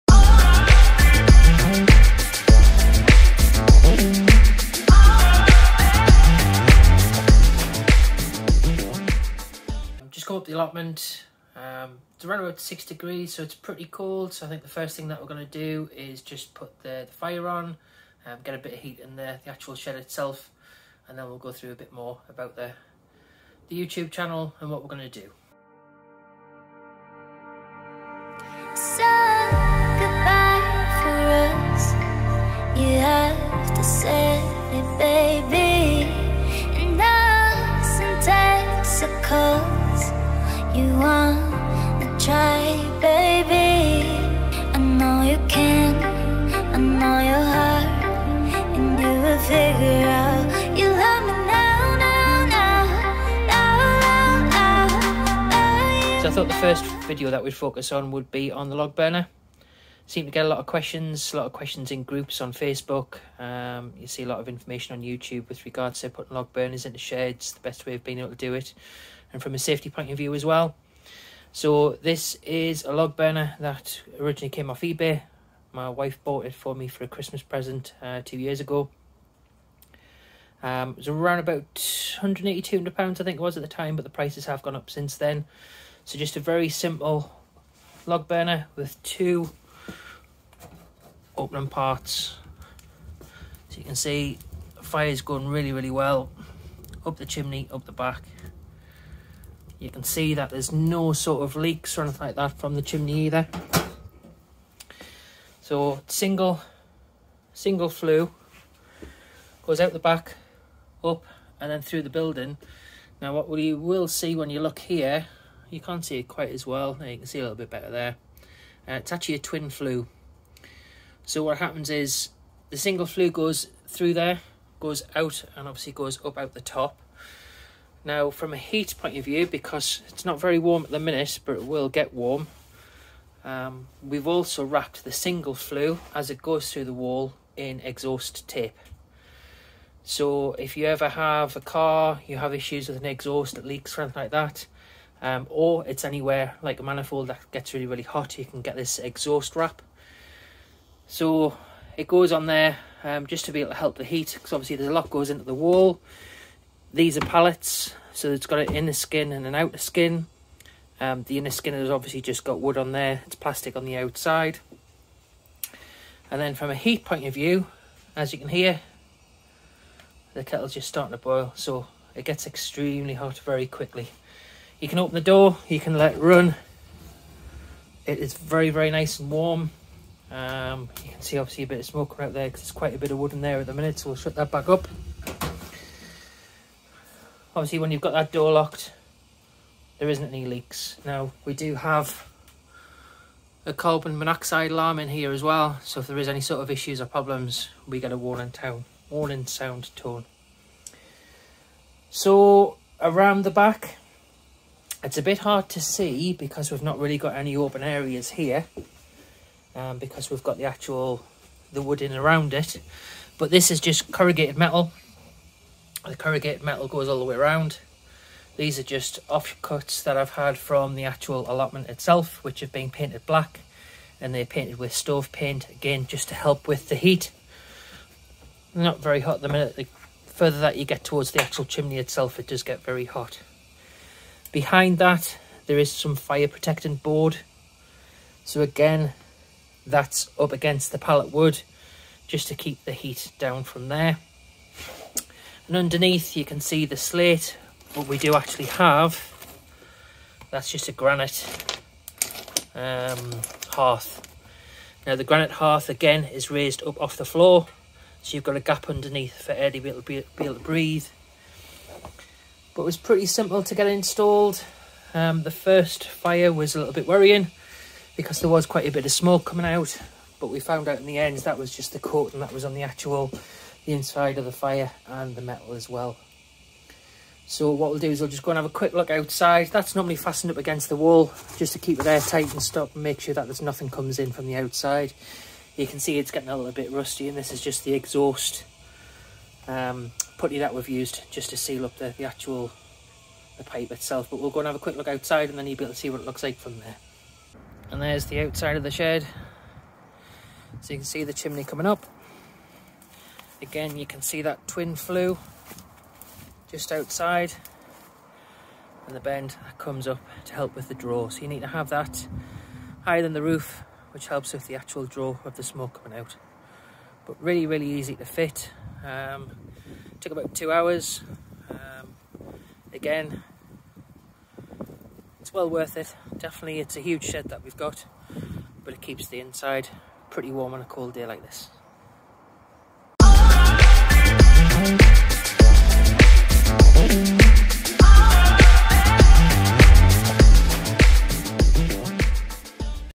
Fitting a wood burner in an allotment shed! DIY Pallet Shed entering @unclewilcoshed of the year
Showing you how you can fit a simple wood burner into an allotment shed with some basic guidelines

Tours around our one off pallet DIY shed build